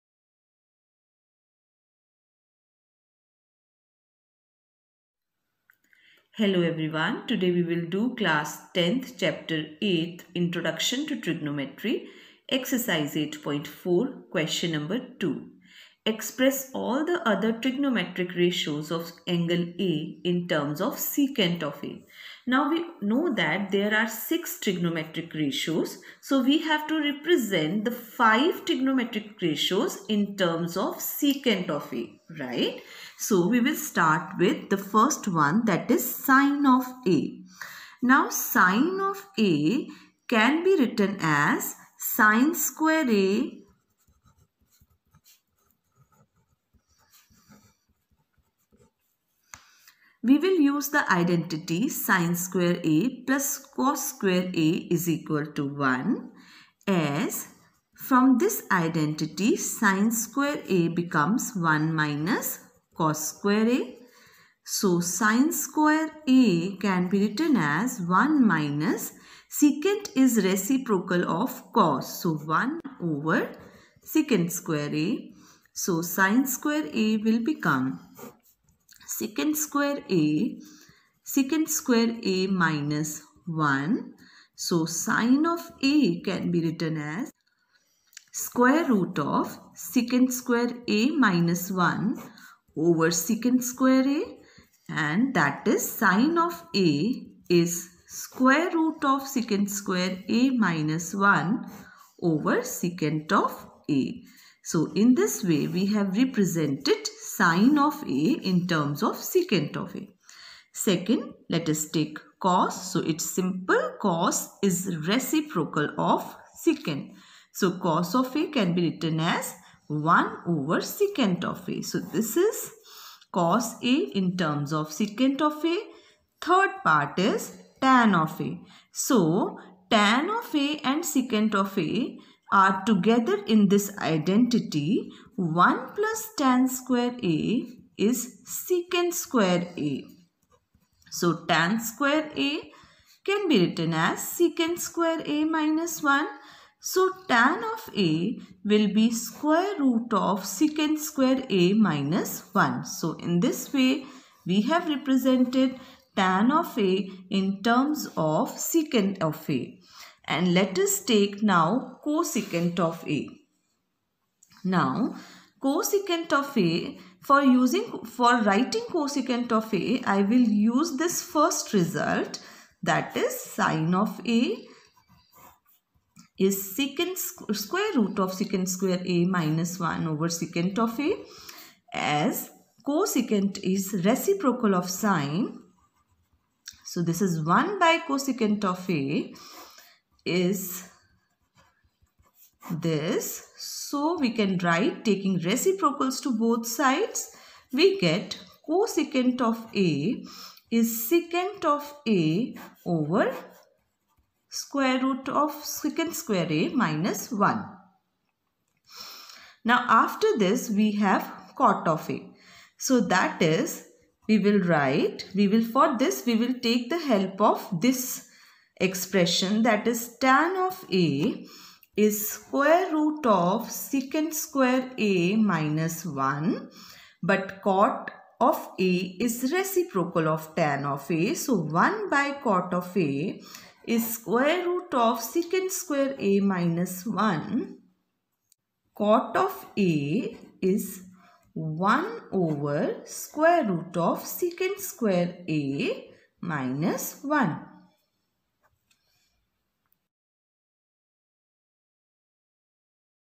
CLASS 10 | EX. 8.4 SUM NO. 2 | INTRODUCTION TO TRIGONOMETRY
Express all the other trigonometric ratios of angle A in terms of Sec A. 5